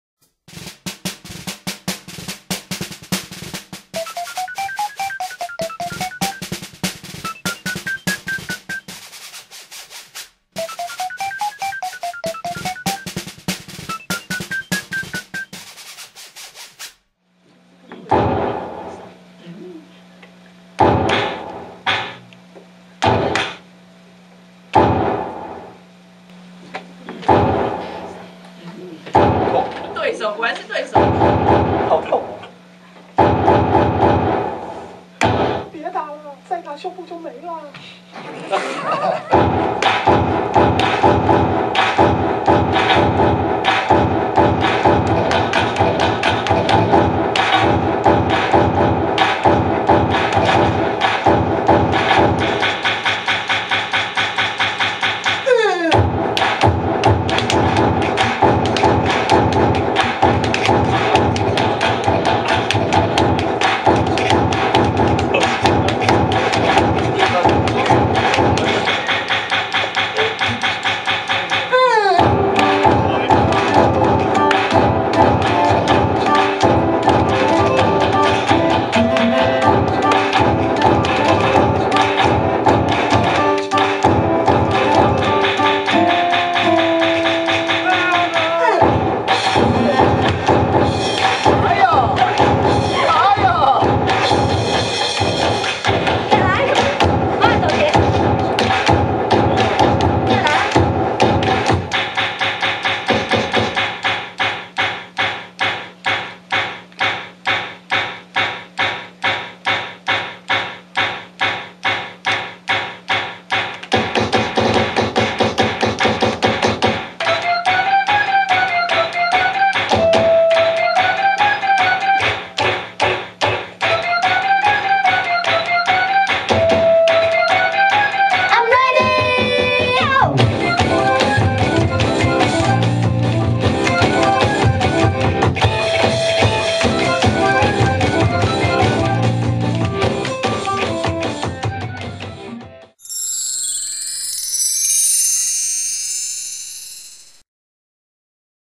戴佩妮野薔薇演唱會【快打炫風-人體打擊秀】彩排實況
戴佩妮野薔薇演唱會【快打炫風-人體打擊秀】彩排實況, 出道十年紀念鉅作【野薔薇演唱會】於2009年台北場演唱會的開場震撼演出 【快打炫風-人體打擊秀,戴佩妮和樂手在身上裝滿觸發式電子開關, 在身體各處連接開關至電腦,7個人身上共有45個開關,同時代表了7種樂器,全台首創的人體打擊秀,花了長達兩個半月的時間練習,現在讓我們來看看戴佩妮以及D Power的練習實況